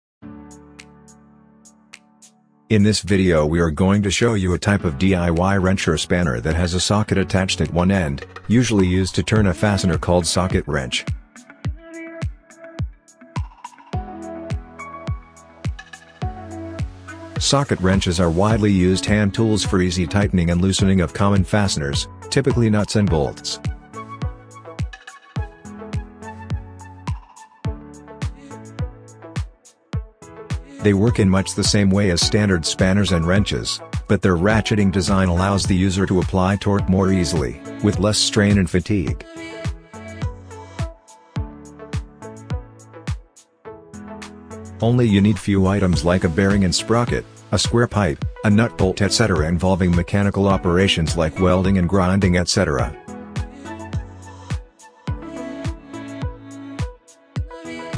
DIY Crafts || how to make a DIY socket wrench or spanner wrench
hi guys||  in this video i am going to show you how to make a socket wrench for your home use  from easy items like bearing, nut bolt and pipe etc. if you like the video plz like and  subscribe